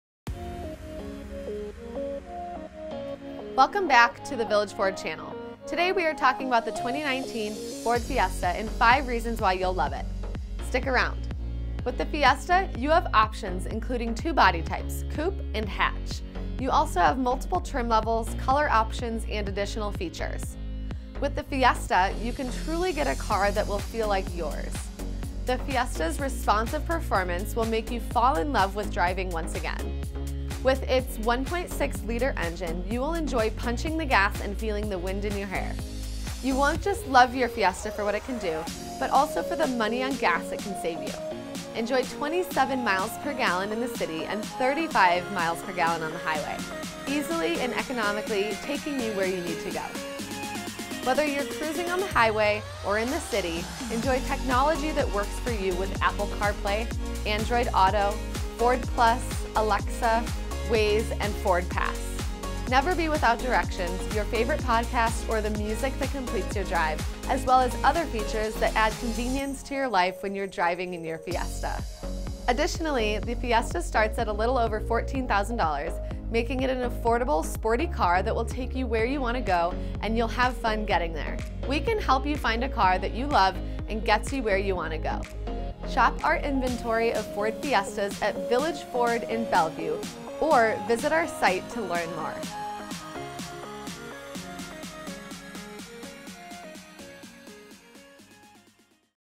2019 Ford Fiesta - Shop Our Belleview Dealership | Village Ford
The Ford Fiesta is the economy vehicle that you will love to drive for its sporty styling, excellent gas mileage, and dependability. Learn more about why you’ll love the Fiesta here.

No matter if you’re looking for your first car or your next favorite car, the Ford Fiesta is a perfect choice. It comes in multiple colors, trim, and styling options that allow you to get the most out of your vehicle no matter where the open road takes you. Get in, get out, and get on the road with a car made for life that is on-the-go. Visit our dealership for competitive prices!